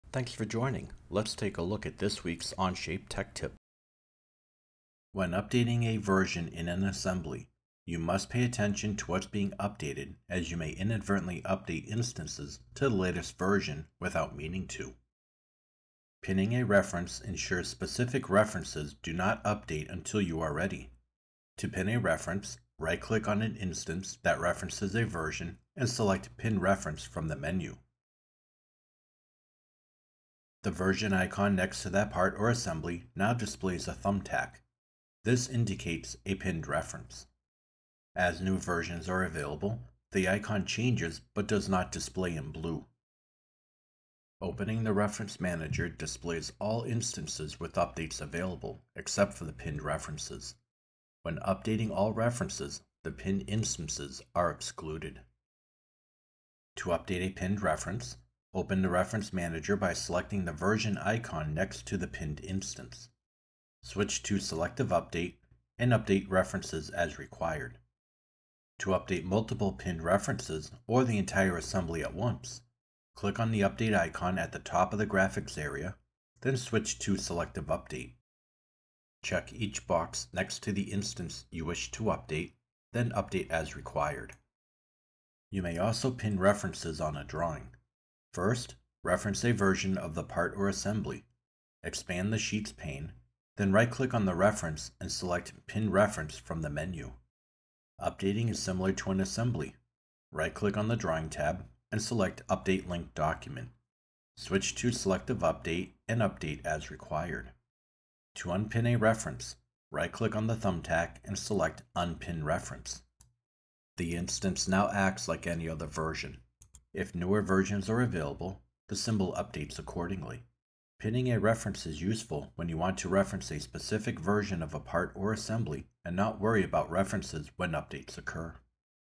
Tech Tip: How to Pin a Reference in an Assembly and Drawing
This Tech Tip will help you learn how to prevent unintended updates from occurring when referencing a version and how to selectively update those pinned references. See the step by step tutorial in the complete blog post